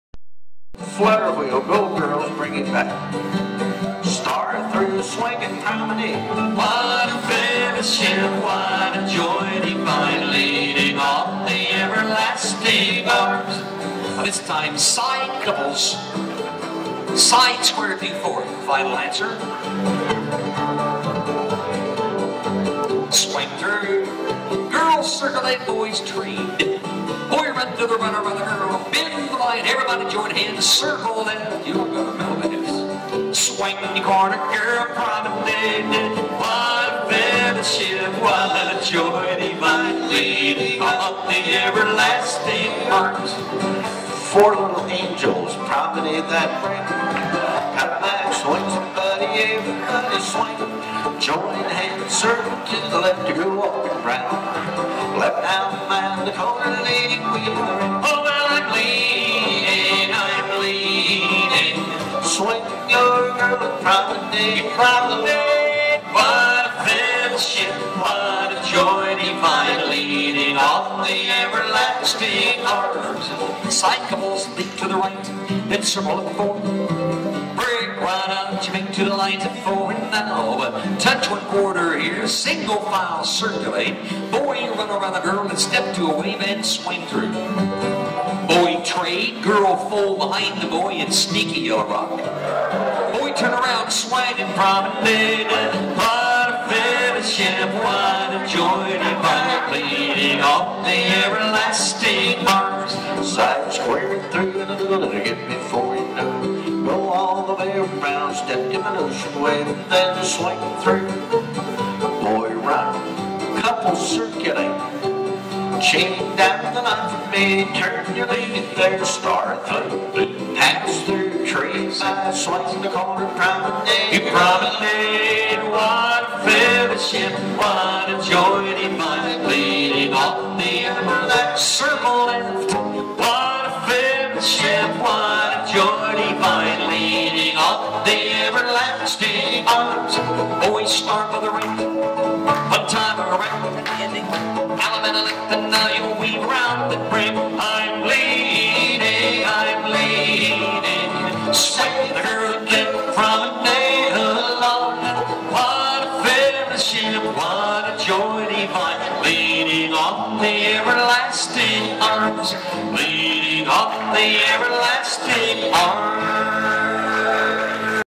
Square Dancing in Broken Bow, Nebraska Modern Western Style with Tom & Jerry VIDEO0033.3gp
Square Dancing in Broken Bow, Nebraska Modern Western Style with Tom Roper & Jerry Junck